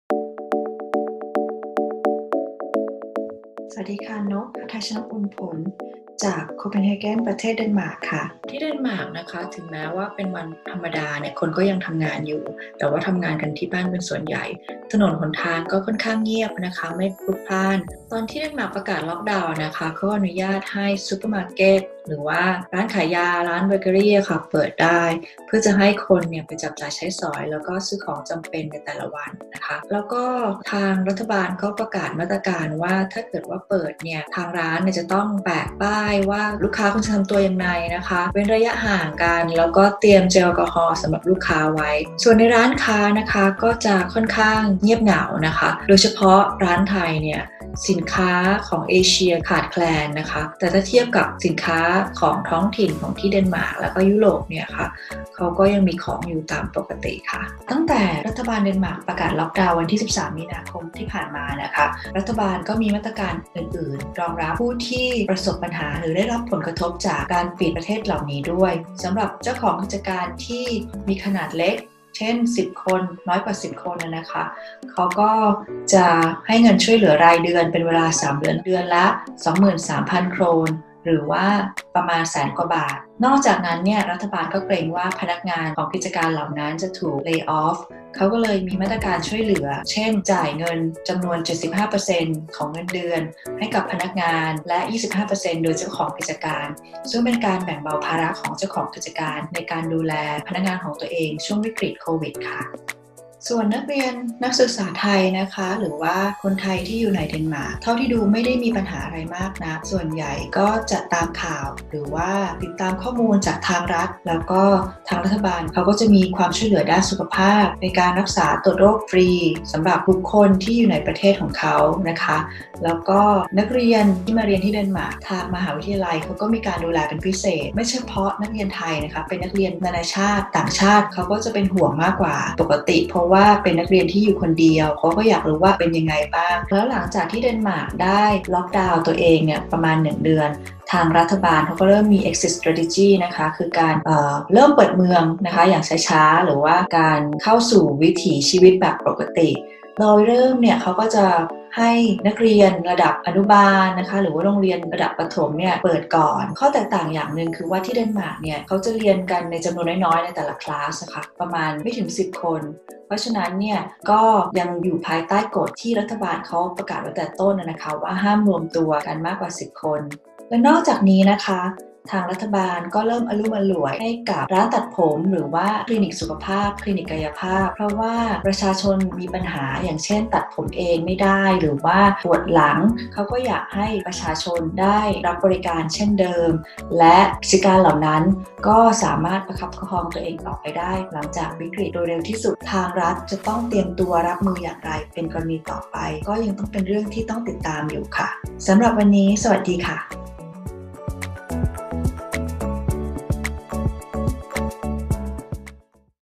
เสียงคนไทย จากโคเปนเฮเกน เดนมาร์ก
คนที่มีครอบครัวหรือเพื่อนไปศึกษาต่อหรือใช้ชีวิตอยู่ประเทศเดนมาร์ก อาจเป็นห่วงว่าคนต่างชาติใช้ชีวิตที่นั่นในช่วงนี้ยังไงบ้าง วันนี้ทีมงานมีคลิปสัมภาษณ์สถานการณ์ล่าสุด จาก ดร. หทัยชนก อุ่นผล หัวหน้าสถาปนิกและนักพัฒนาแอปพลิเคชั่น เมือง Copenhagen ประเทศ Denmark มาให้ชมกัน ติดตามได้จากรายงานพิเศษ